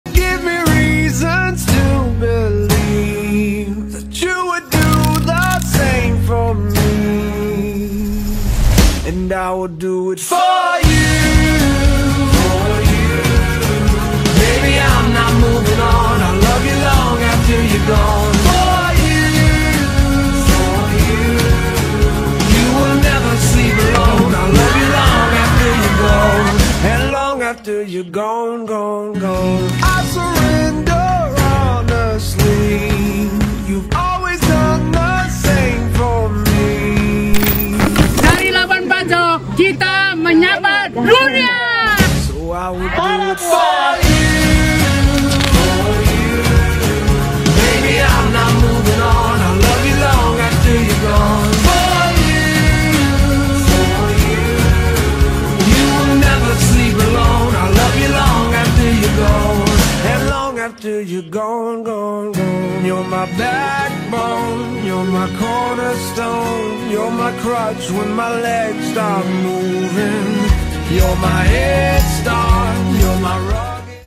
After Movie Picnic Over The Hill POTH Vol 1 Day 2
Picnic Over The Hill (POTH) adalah owned event milik Floratama Production yang dilaksanakan di Kawasan ParaPuar, Labuan Bajo milik Badan Pelaksana Otorita Labuan Bajo Flores (BPOBF). Pertama kali dilaksanakan pada 11 November dan bekerjasama dengan beberapa komunitas ekonomi kreatif lokal. POTH Vol 1 kali ini turut menghadirkan beberapa artis nasional seperti  @siletopenup   dan  @AldoLonga_  sebagai headliners pada event kali ini.